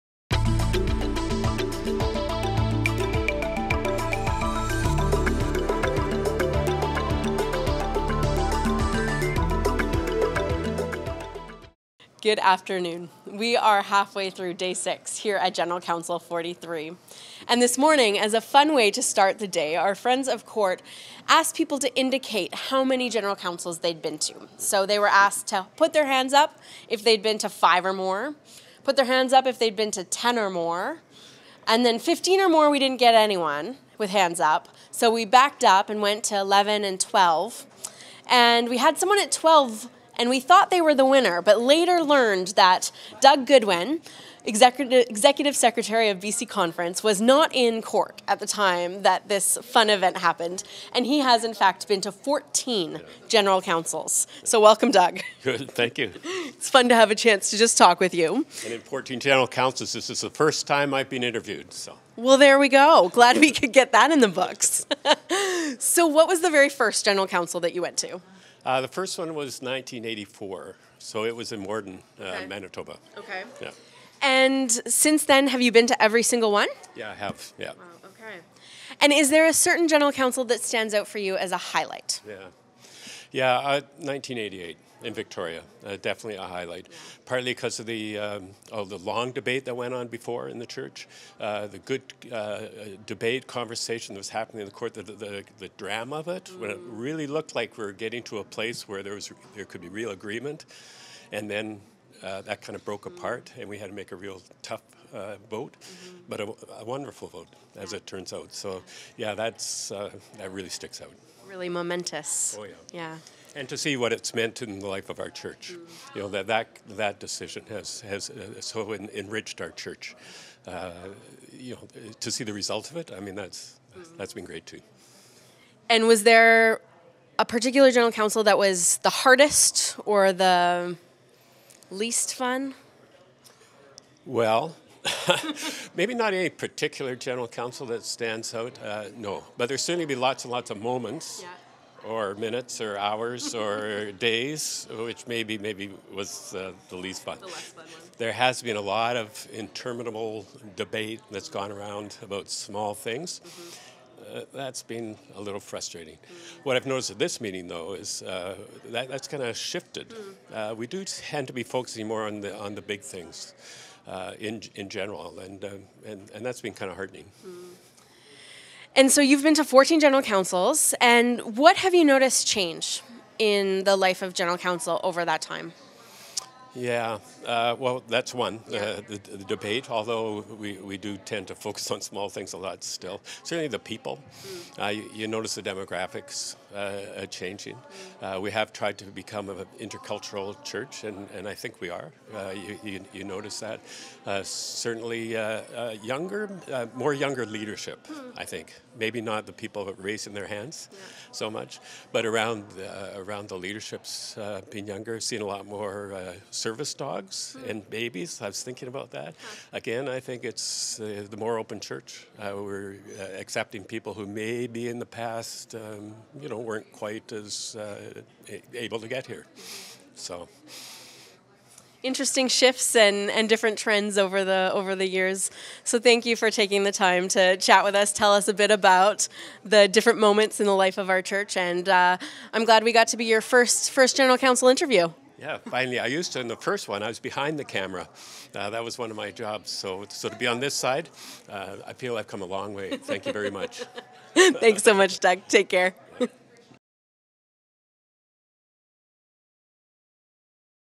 Rev. Doug Goodwin on His 14th General Council
Rev. Doug Goodwin reflects to GC43 reporter Lauren Hodgson on his experience of his 14th General Council. Rev. Goodwin is said to have attended the most General Councils of any commissioner at GC43.